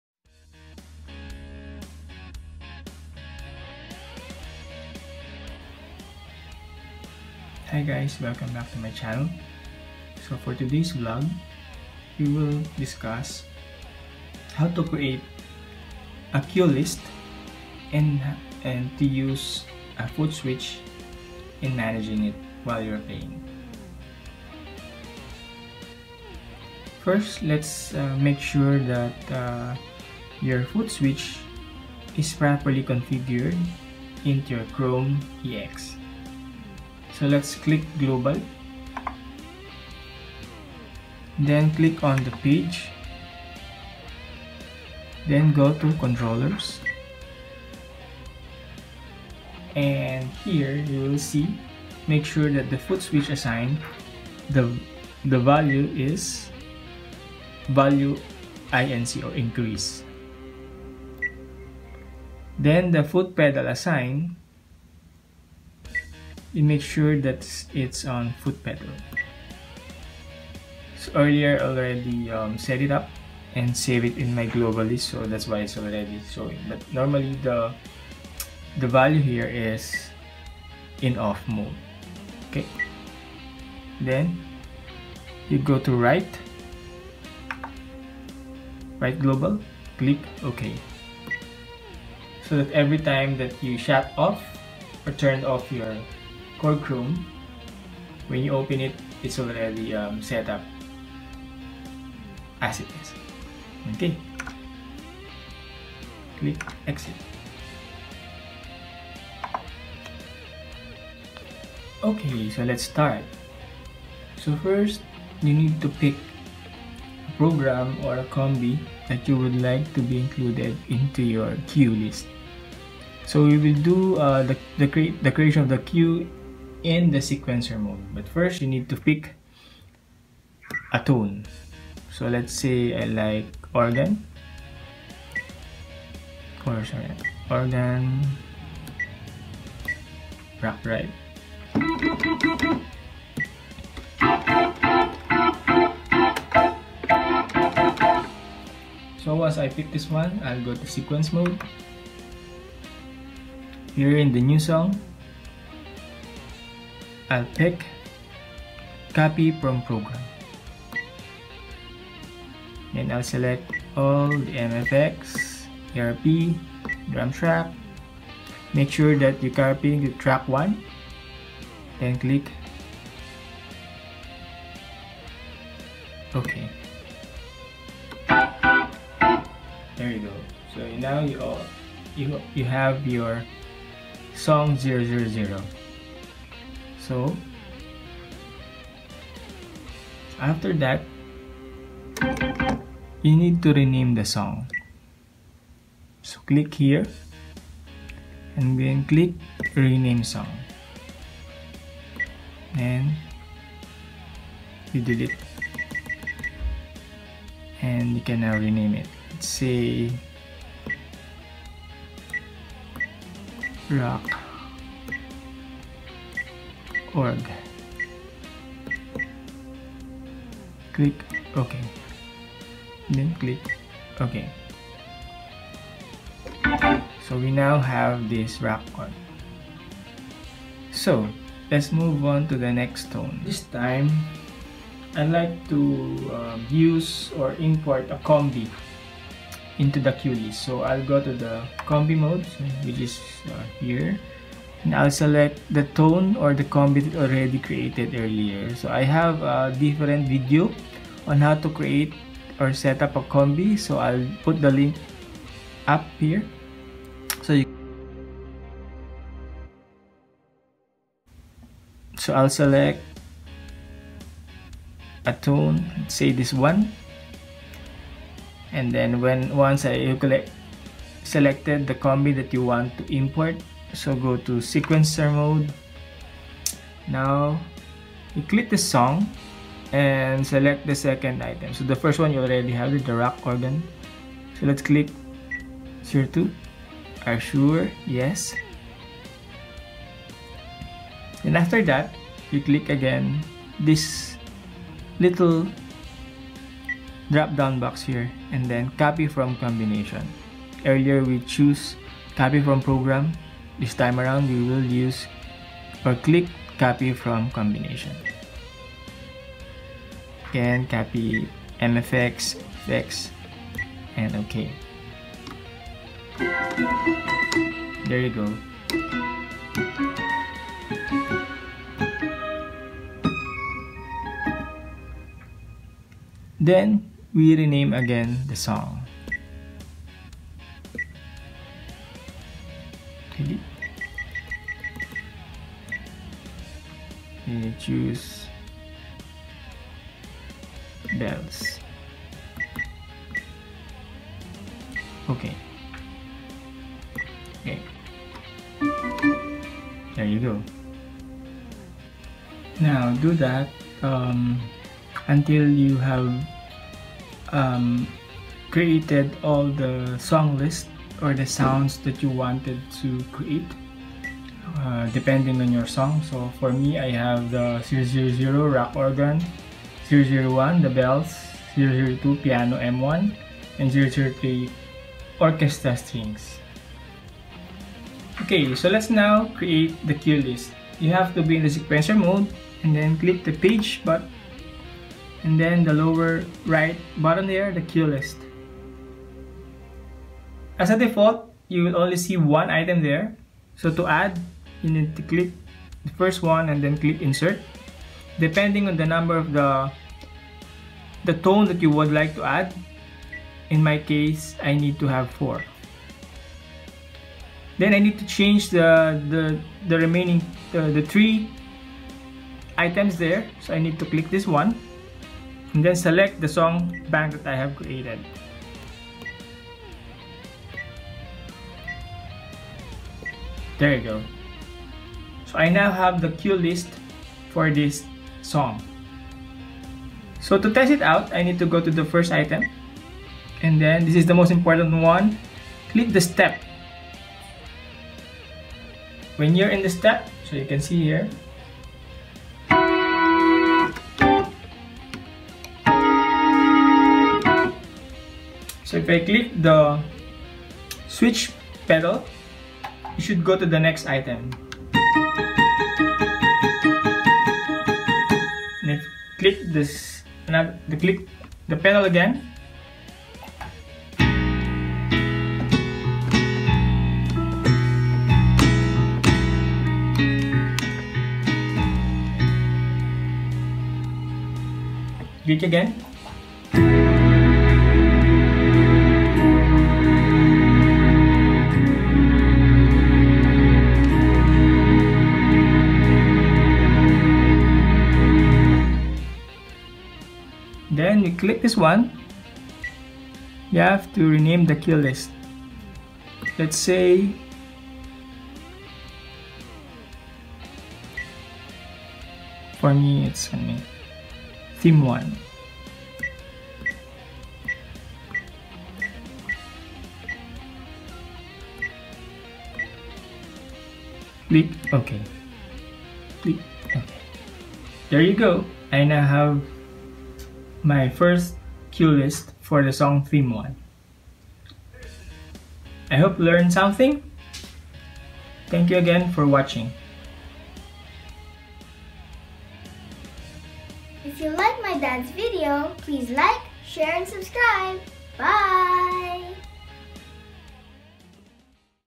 Korg Krome Cue List and Foot Switch Tutorial
If you like the video, please click that like button and help me create more informative, entertaining, and engaging videos.

How to create a cue list and using foot switch tutorial. - provides the keyboard player handsfree switching of Programs and Combi during actual performance in a linear pattern